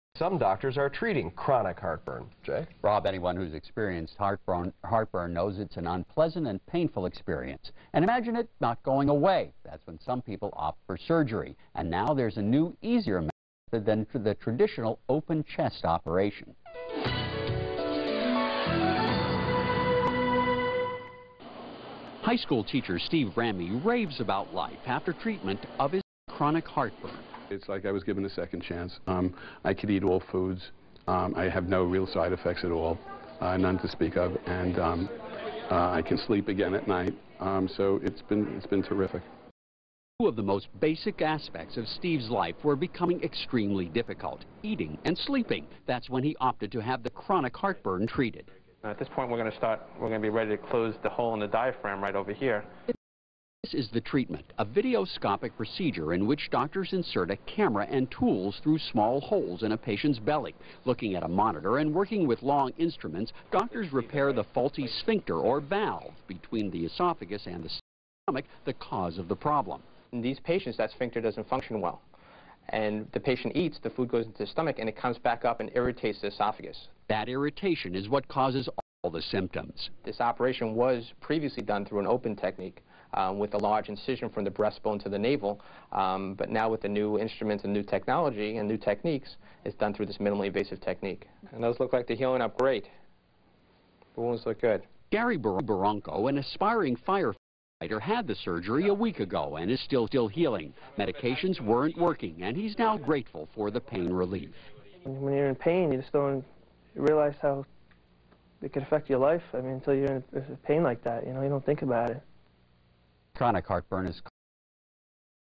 Dr. Garber on ABC7 Talking About Laparoscopic GERD Surgery
Dr. Garber on ABC7 Talking About Laparoscopic Gastroesophageal Reflux Disease Surgery.